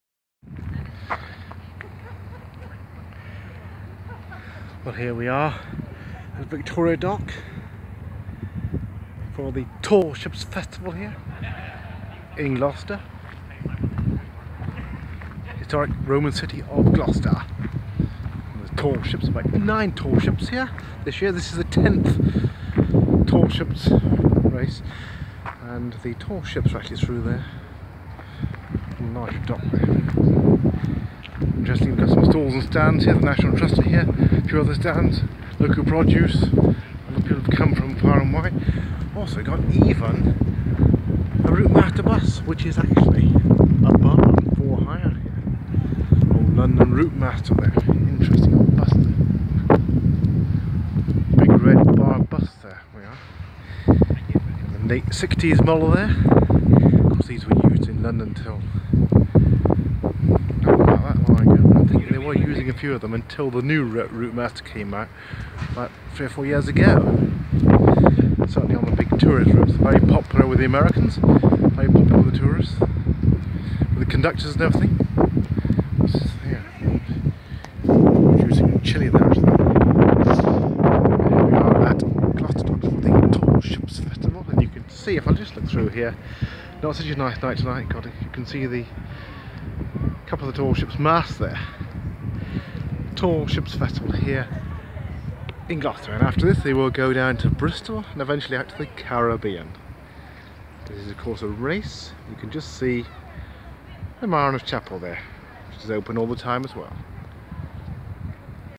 Tall ships festival 2017
Various exhibits at the Tall ships festival including a RouteMaster bus fitted out as a bar!